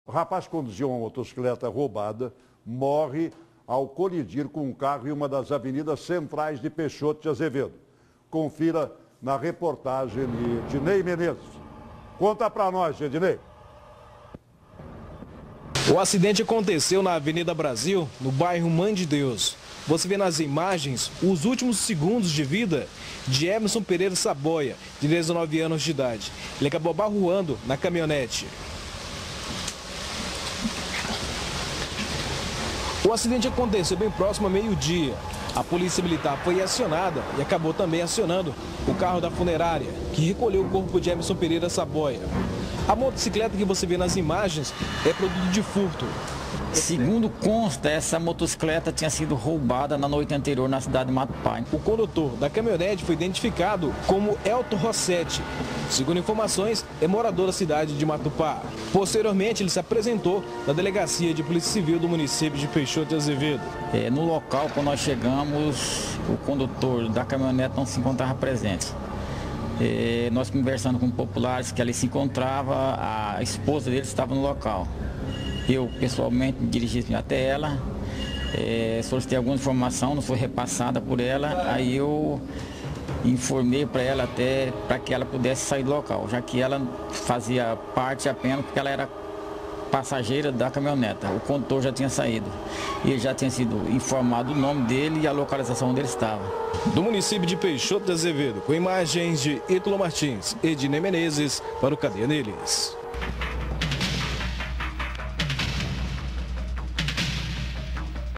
Jovem morre ao colidir motocicleta roubada em veículo
O acidente aconteceu na cidade de Peixoto de Azevedo.